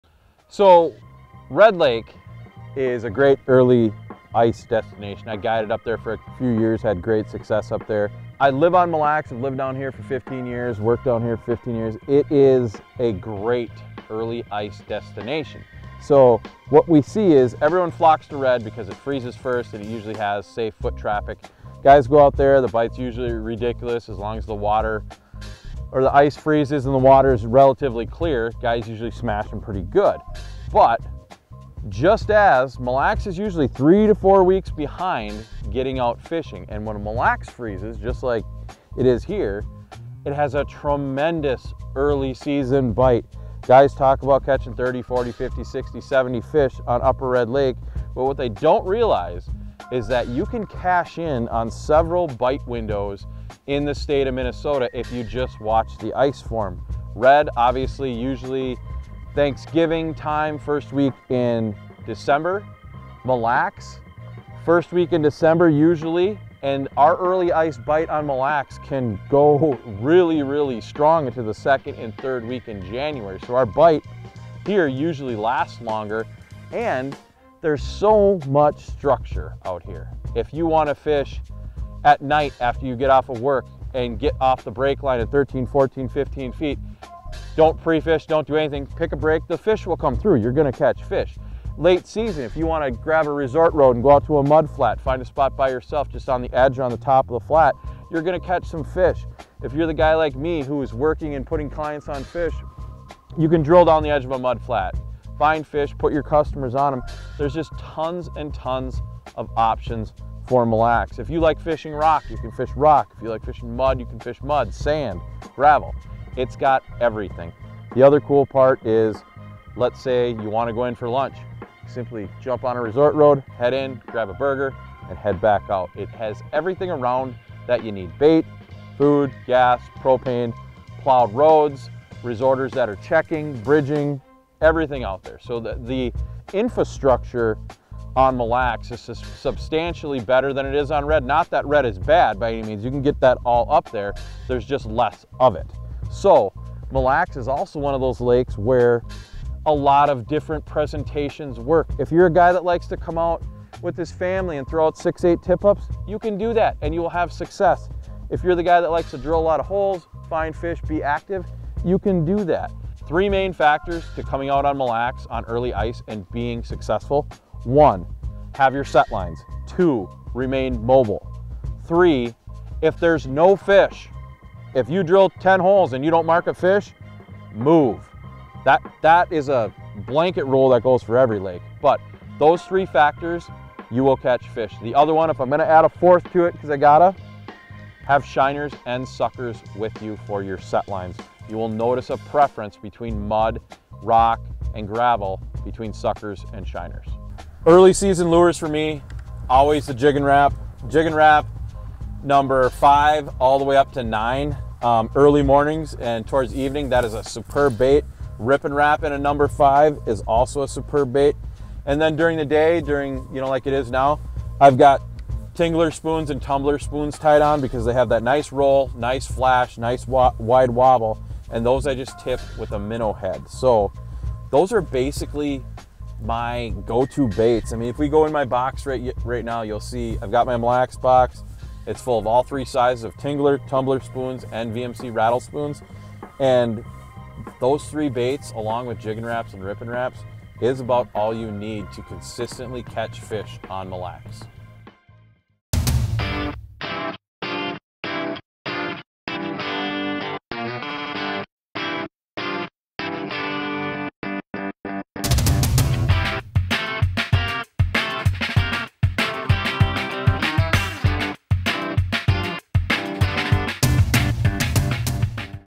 Red Lake vs. Mille Lacs (for FIRST ICE)
Minnesota guide Brad Hawthorne shares his two cents on the early season bite windows on Mille Lacs and Red Lake, two lakes he's spent plenty of time guiding on. He also offers some bonus tips for whackin' walleye on Mille Lacs.

Don't forget to grab your ice fishin' gear at your local Fleet Farm store or online: